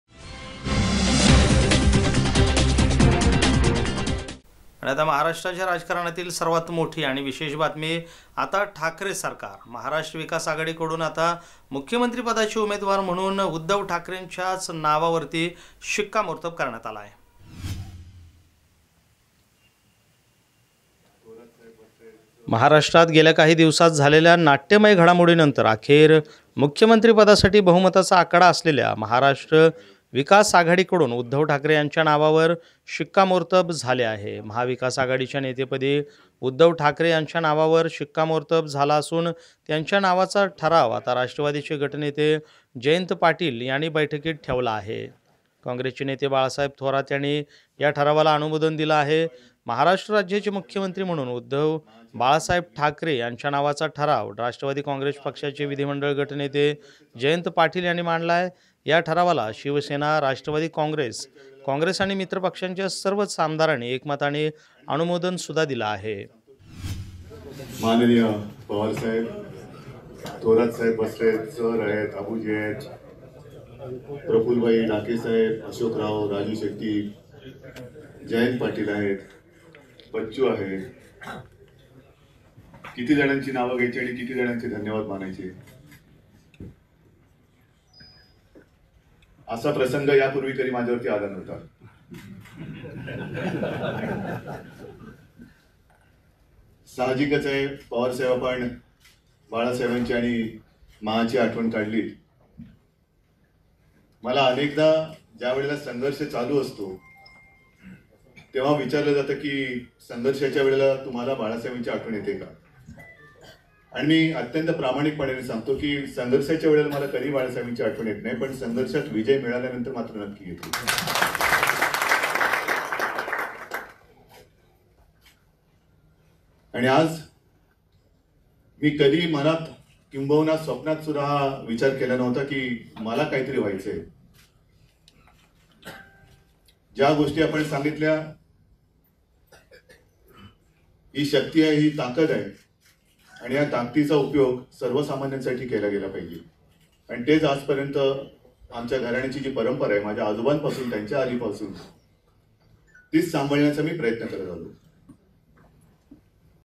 आता ठाकरे सरकार, 'महाराष्ट्र विकास आघाडी'कडून मुख्यमंत्रीपदाचे उमेदवार म्हणून
आता ठाकरे सरकार, 'महाराष्ट्र विकास आघाडी'कडून मुख्यमंत्रीपदाचे उमेदवार म्हणून उद्धव ठाकरेंच्या नावावर शिक्कामोर्तब.@SBN मराठी.